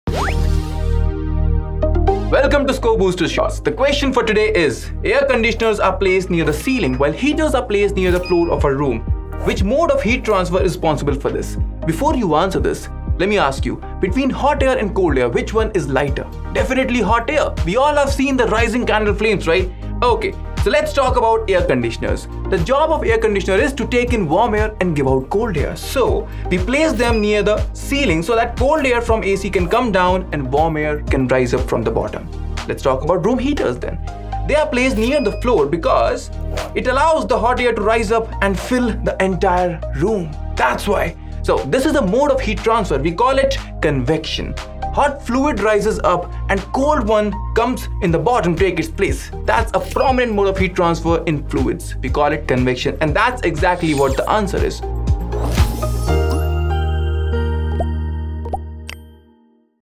Why are Air Conditioners near the Ceiling and Heaters Placed near the Floor | BYJU'S - Class 7
🔷 BYJU's Mini learning Program is now FREE 🤩 go for it!!!
Use Code- YTFREE
Last 8 days to avail YTFREE ⏰

Have you ever wondered why ACs are placed near the ceiling and room heaters near the floor?
Saurabh Sir, your BYJU'S educator, is here with another Score Booster in which he answers this question. In this session, Saurabh Sir talks about convection, a mode of heat transfer that plays an important role in determining the placements of ACs and room heaters.
Here is a question asked by Saurabh Sir in this session. Can you answer this question?
Question: Air conditioners are placed near the ceiling while heaters are placed near the floor of a room. Which mode of heat transfer is responsible for this?​
To find out the right answer, watch this video.